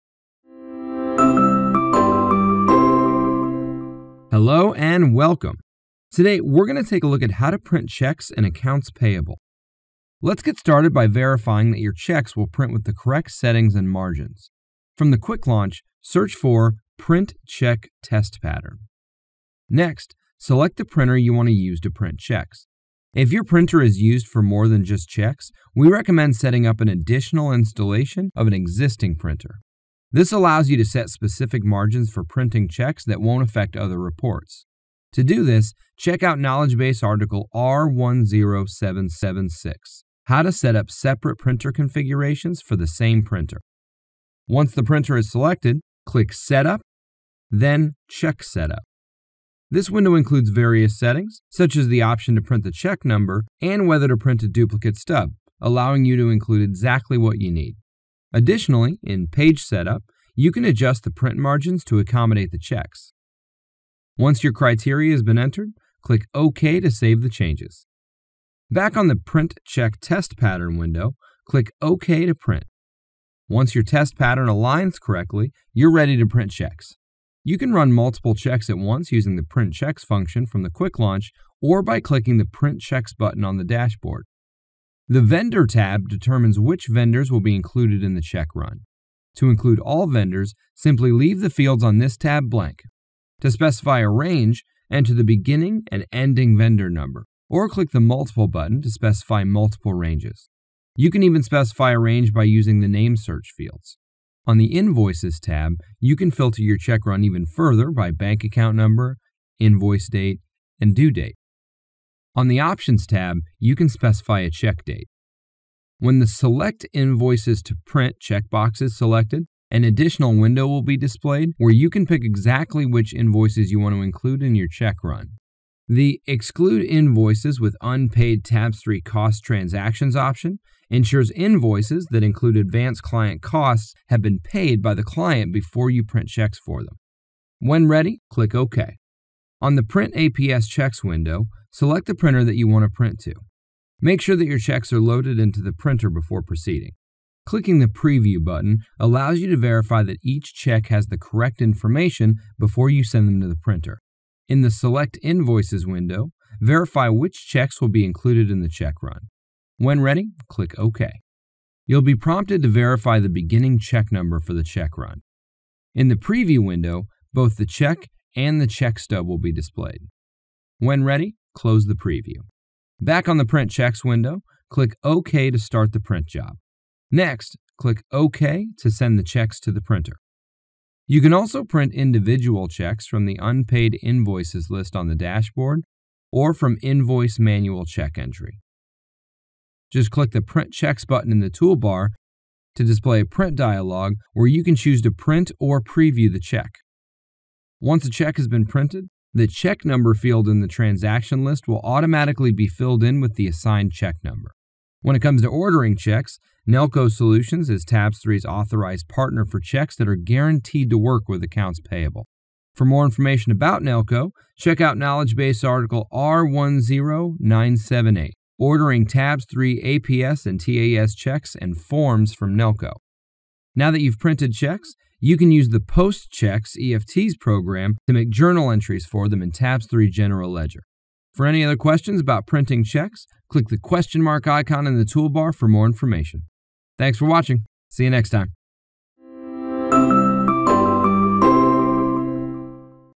Printing Checks in Accounts Payable
See how easy Tabs3 Accounts Payable makes it to print checks from your firm’s bank accounts.